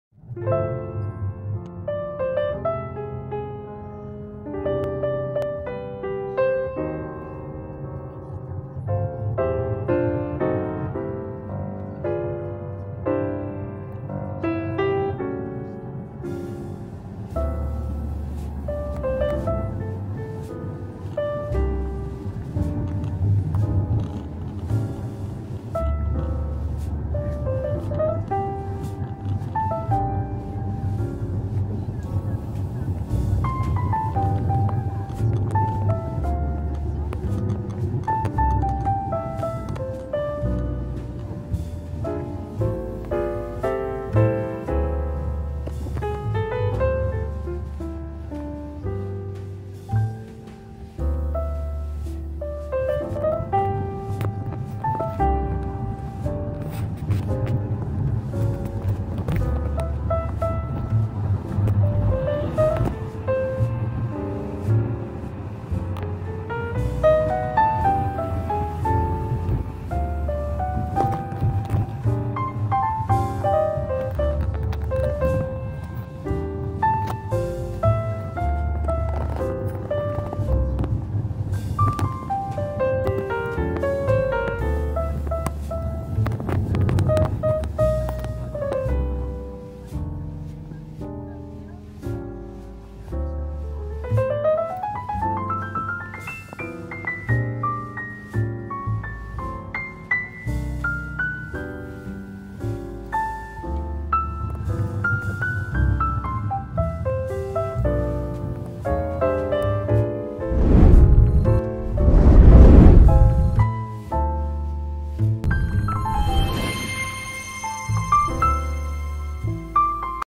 Wet and Dark Vienna 🇦🇹
It was still 4:30 in the afternoon but it was already dark in Vienna and the droplets of rain added to the beauty of the city.